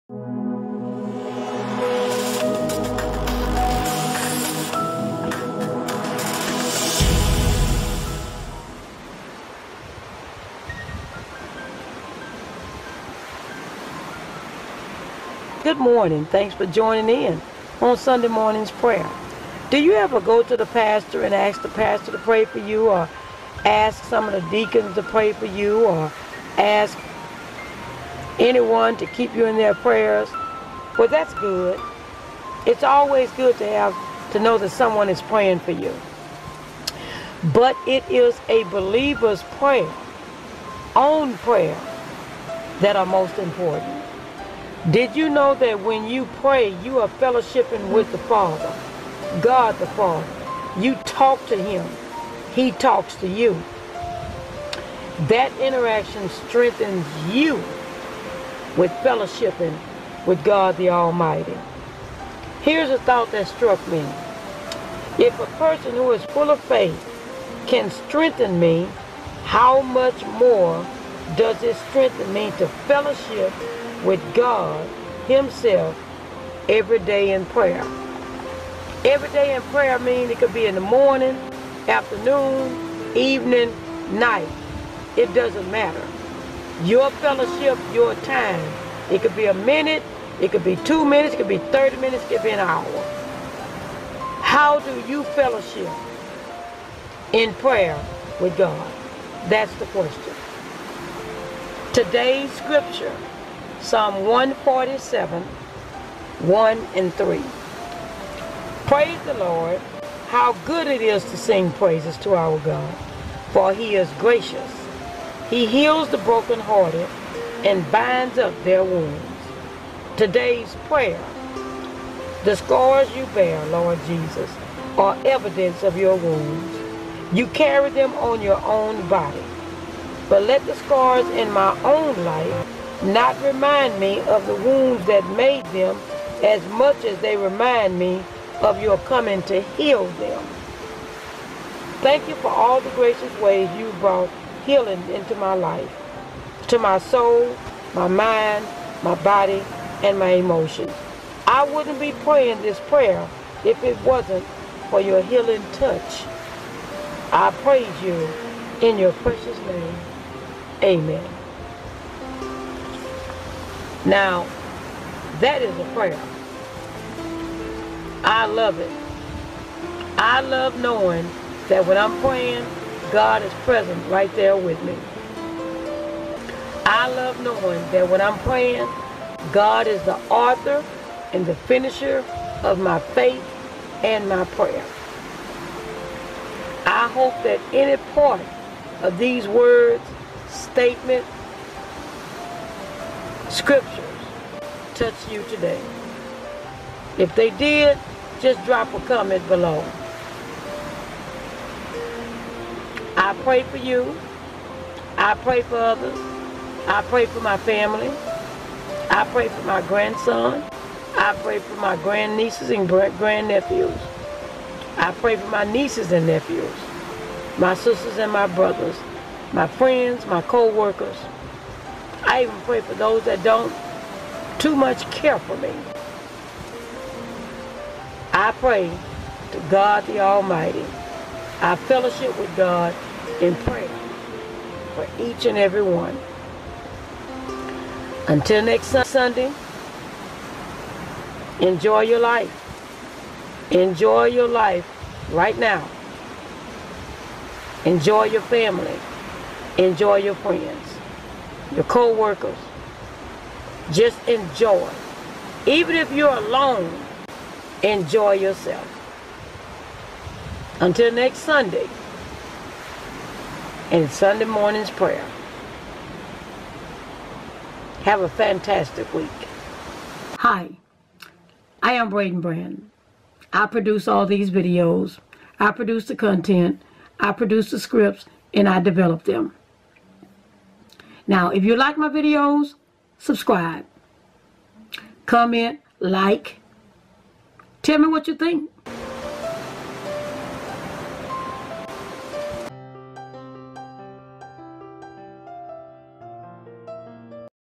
Sunday Morning Prayer How do I learn to pray for myself. Braden Brand
Sunday Morning Prayer. How do I learn to pray for myself. Braden Brand.  I've learned that sometimes when you need others to pray for you they are not available, so I had to learn to pray for myself,

S4/Vlog 232